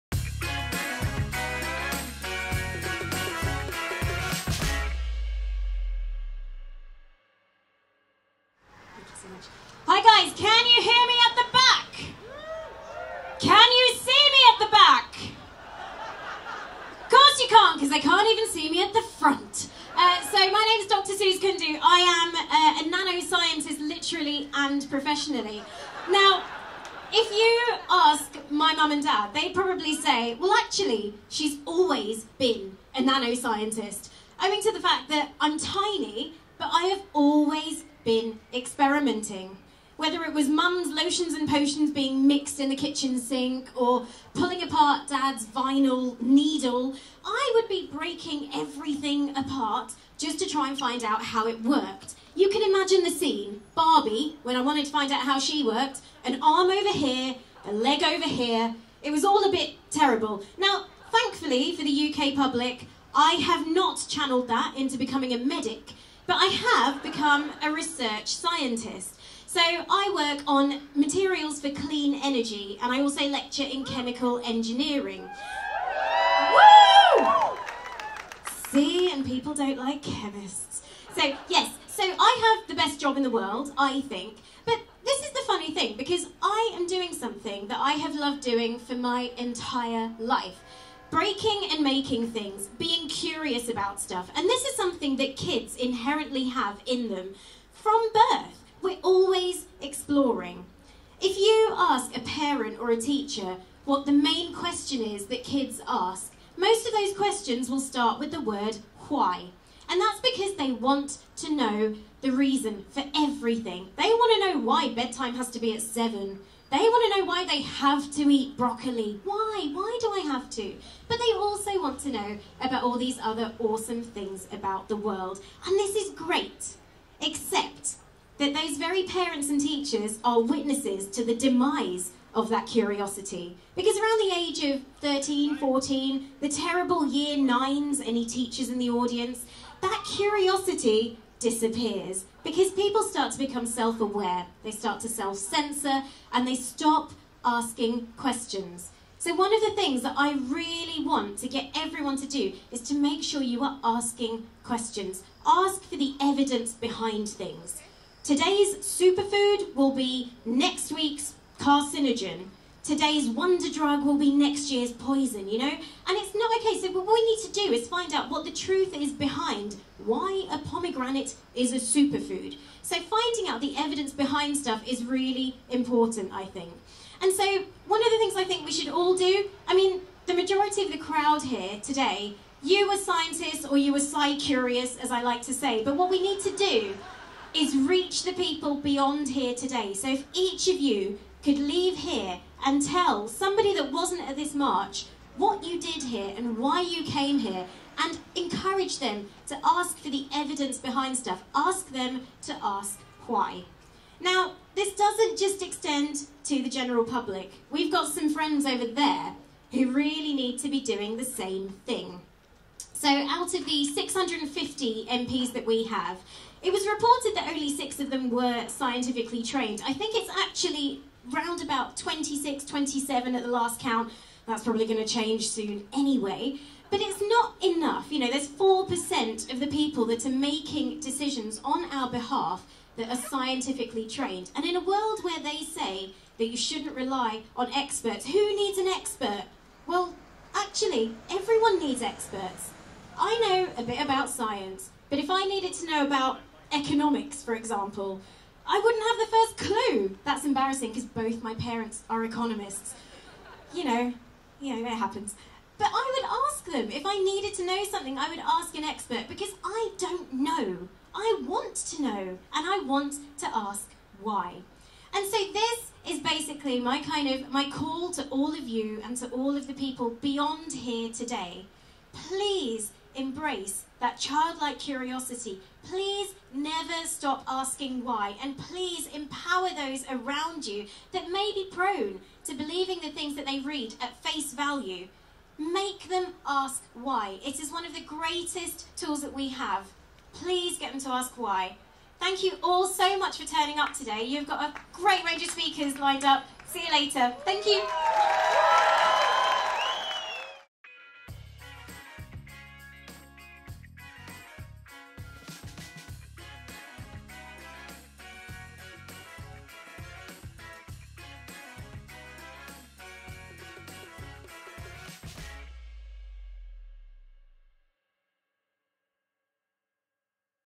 Suze Kundu Speech - March for Science London
Dr Suze Kundu's, nanoscientist, speech as part of the rally in Westminster at the end of the march in London

The Cosmic Shambles Network is proud to be the official legacy partner of the March for Science.